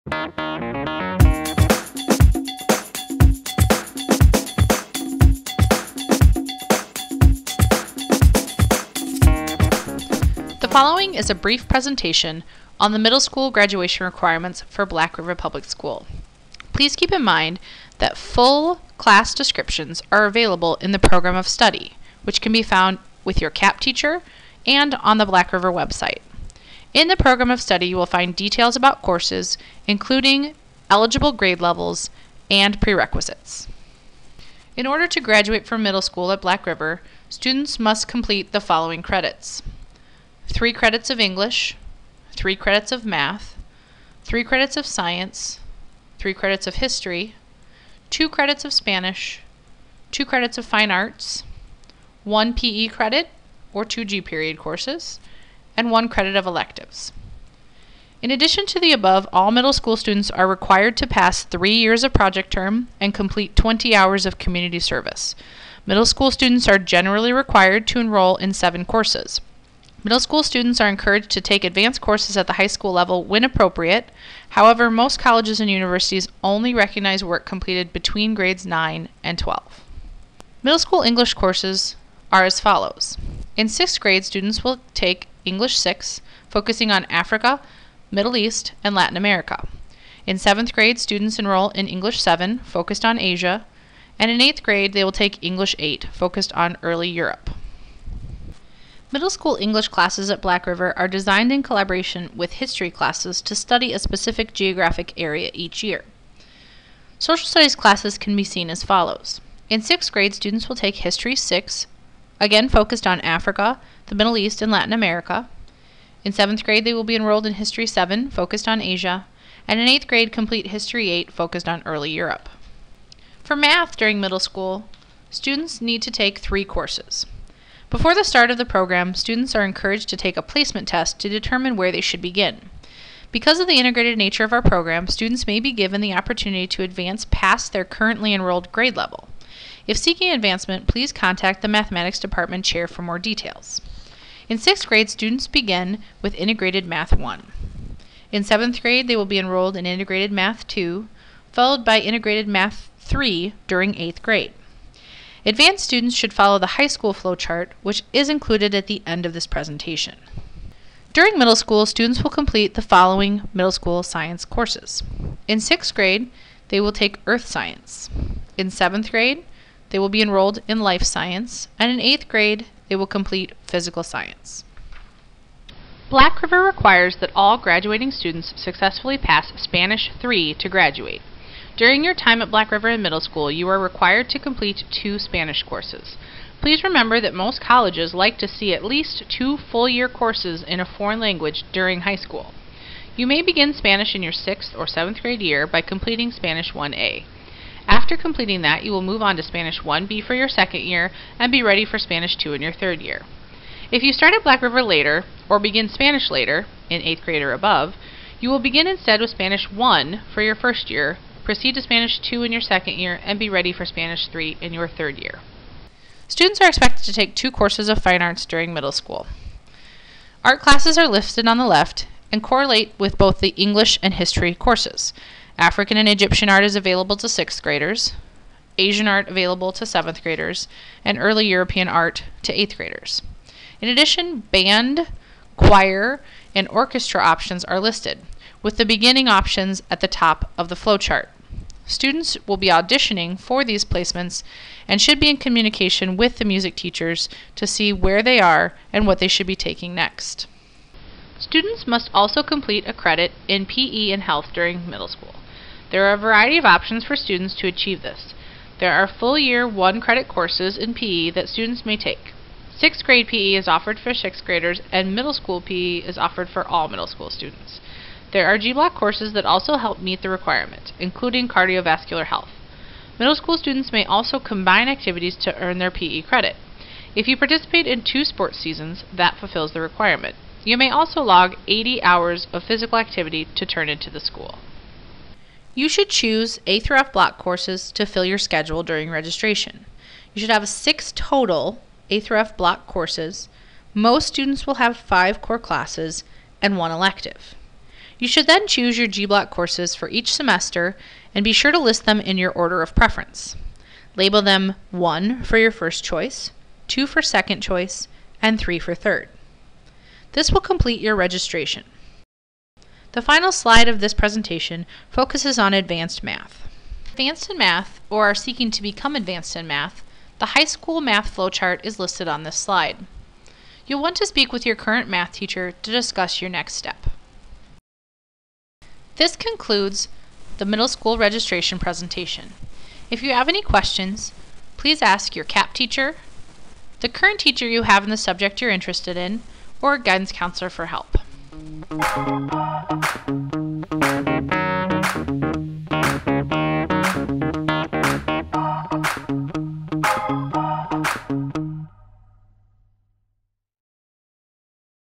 MS Course Registration 2023final
Brief video describing MS graduation requirements for Black River Public School.